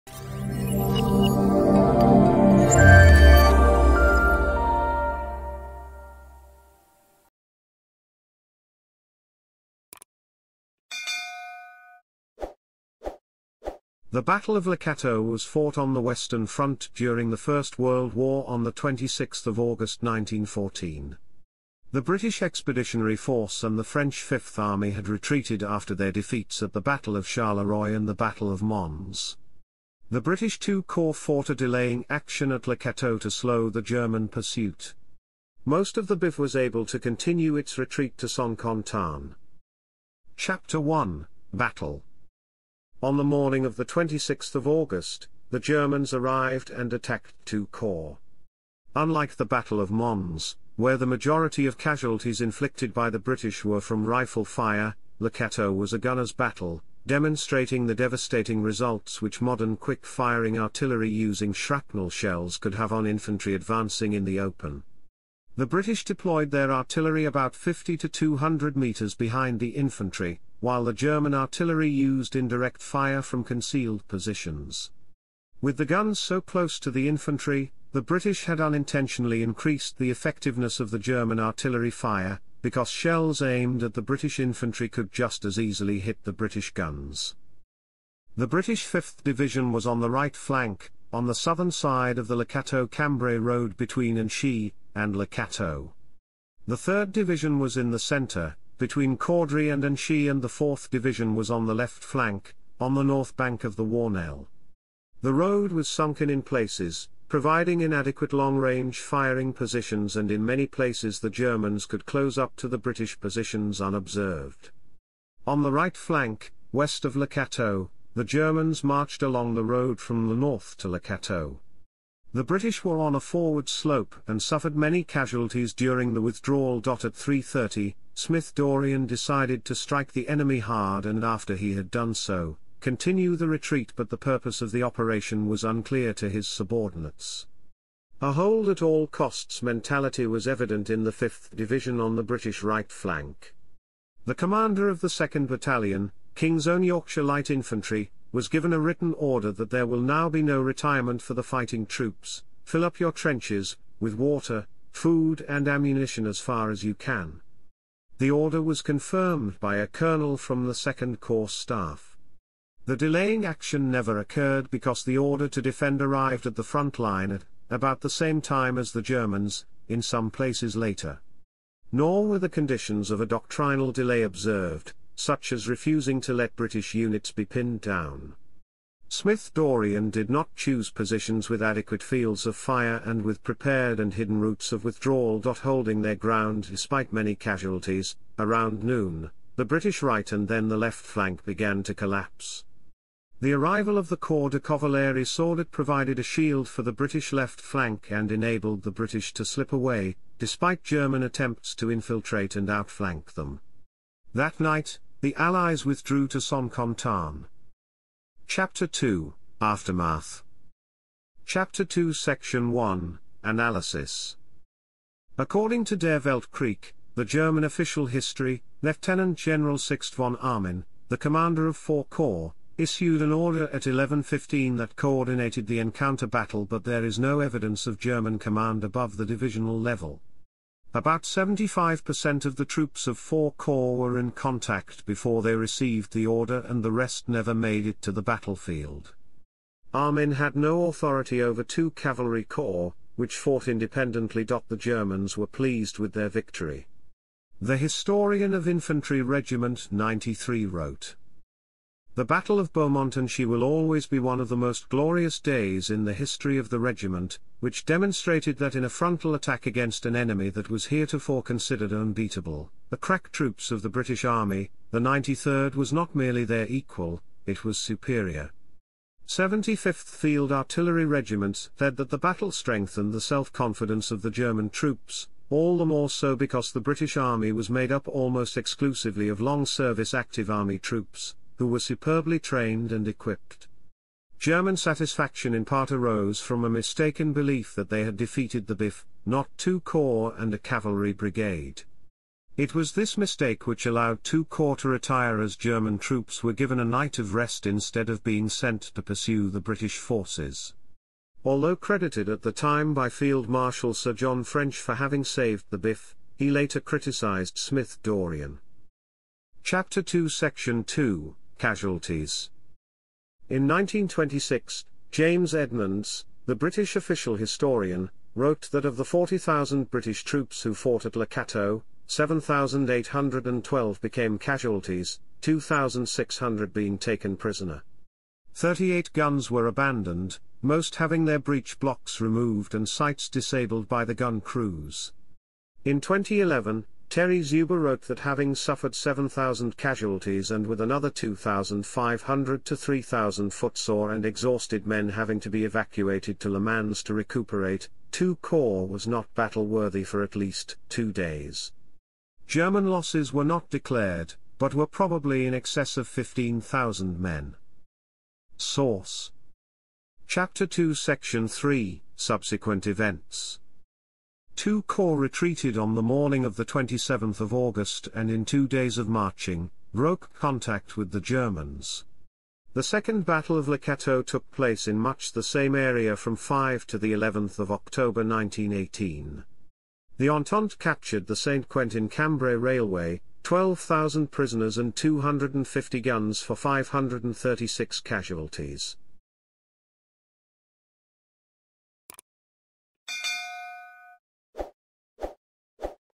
Battle of Le Cateau - Wikipedia article
Video summary:
The Battle of Le Cateau was fought on the Western Front during the First World War on 26 August 1914. The British Expeditionary Force (BEF) and the French Fifth Army had retreated after their defeats at the Battle of Charleroi (21–23 August) and the Battle of Mons (23 August). The British II Corps fought a delaying action at Le Cateau to slow the German pursuit. Most of the BEF was able to continue its retreat to Saint-Quentin.